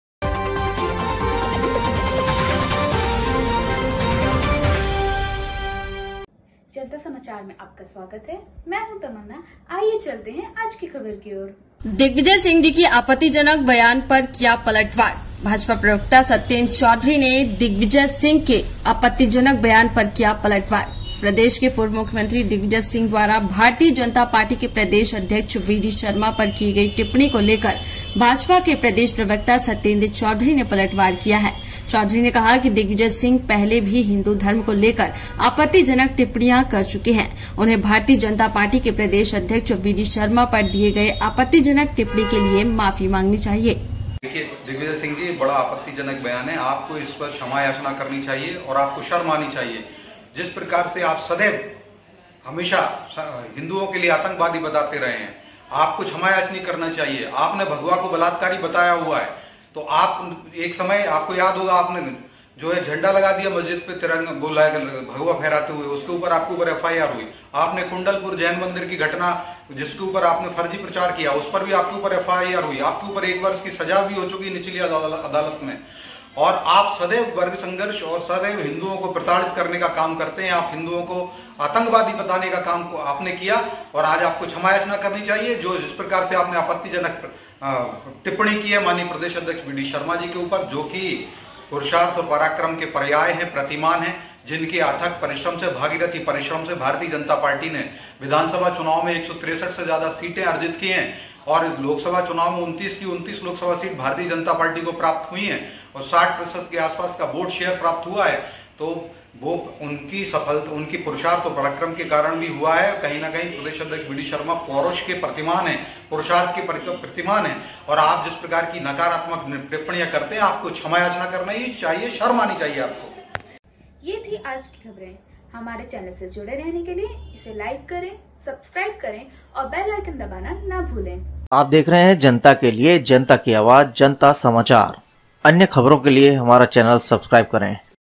BJP प्रवक्ता सत्येंद्र चौधरी ने दिग्विजय सिंह पर किया पलटवार7/9/24JantaSamachar.news#viral#hindinews
# Bhajpa pravakta Satyendra Chaudhari Ne Digvijay Singh per Kiya paltvar
BJP प्रवक्ता सत्येंद्र चौधरी ने दिग्विजय सिंह पर किया पलटवार